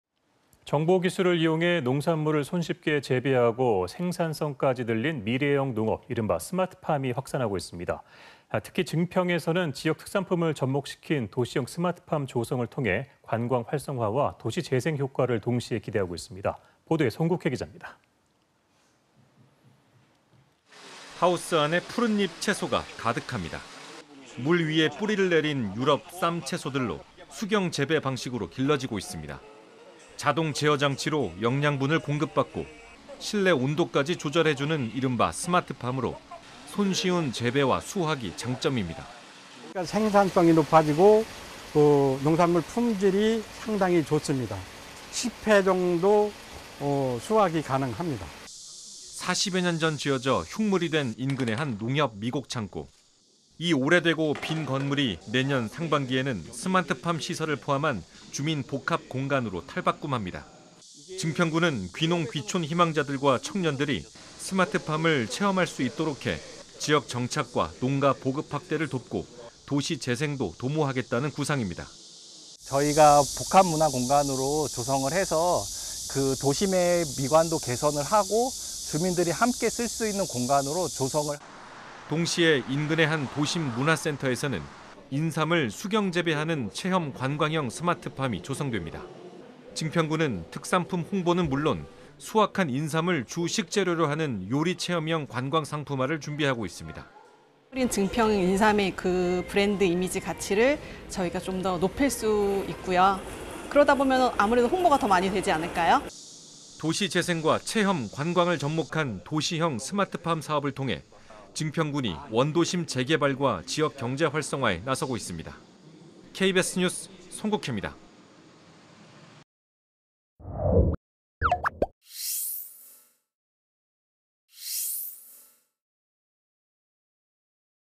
증평에 도시형 스마트팜 조성…“관광·도시재생 기대” / KBS 2023.08.21.
[앵커]

 정보 기술을 이용해 농산물을 손쉽게 재배하고 생산성까지 늘린 미래형 농업, 이른바 스마트팜이 확산하고 있습니다.

 특히 증평에서는 지역 특산품을 접목시킨 도시형 스마트팜 조성을 통해 관광 활성화와 도시재생 효과를 동시에 기대하고 있습니다.

 보도에 송국회 기자입니다.

 [리포트]

 하우스 안에 푸른 잎 채소가 가득합니다.

 물 위에 뿌리를 내린 유럽 쌈 채소들로 수경재배방식으로 길러지고 있습니다.

 자동 제어 장치로 영양분을 공급받고, 실내 온도까지 조절해주는 이른바 '스마트팜'으로 손쉬운 재배와 수확이 장점입니다.

 [손현성/증평군 도안면 : "생산성이 높아지고 농산물 품질이 상당히 좋습니다. (연중) 10회 정도 수확이 가능합니다."]

 40여 년 전 지어져 흉물이 된 인근의 한 농협 미곡 창고.

 이 오래되고 빈 건물이 내년 상반기에는 스마트팜 시설을 포함한 주민 복합 공간으로 탈바꿈합니다.

 증평군은 귀농·귀촌 희망자들과 청년들이 스마트팜을 체험할 수 있도록 해 지역 정착과 농가 보급 확대를 돕고, 도시 재생도 도모하겠다는 구상입니다.

 [김현식/증평군 공원녹지팀 주무관 : "저희가 복합문화공간으로 조성해서 도심 미관도 개선하고 주민들이 함께 쓸 수 있는 공간으로 조성을…."]

 동시에 인근 한 도심 문화센터에서는 인삼을 수경 재배하는 체험·관광형 스마트팜이 조성됩니다.

 증평군은 특산품 홍보는 물론 수확한 인삼을 주 식재료로 하는 요리 체험형 관광 상품화를 준비하고 있습니다.

 [이윤정/증평군 인삼특작팀장 : "증평 인삼의 브랜드 이미지 가치를 좀 더 높일 수 있고요. 그러다 보면 아무래도 홍보가 더 많이 되지 않을까요."]

 도시 재생과  체험, 관광을 접목한 도시형 스마트팜 사업을 통해 증평군이  원도심 재개발과 지역경제 활성화에 나서고 있습니다.

 KBS 뉴스 송국회입니다.

 촬영기자:김장헌